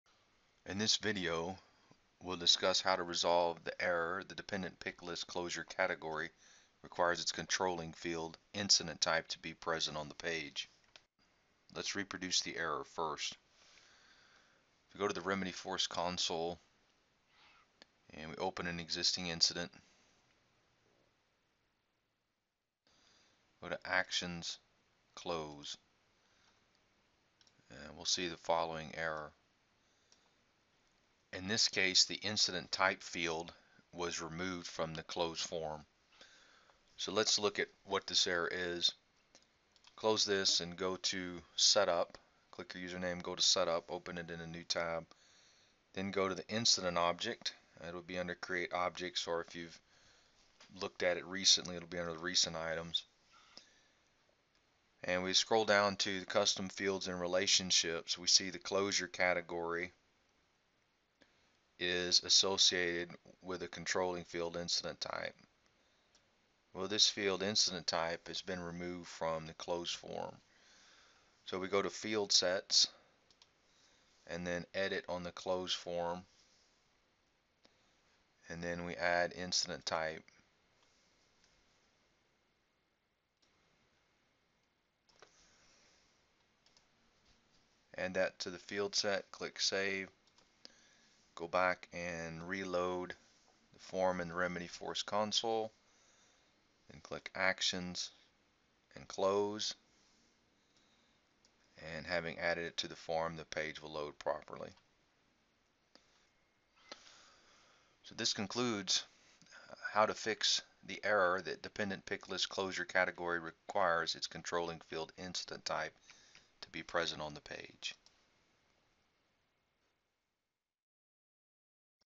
Fixing Remedyforce Dependent Picklist Required Controlling Field Errors
How to Fix Error: The dependent picklist 'Closure Category' requires its controlling field 'Incident Type' to be present on the page.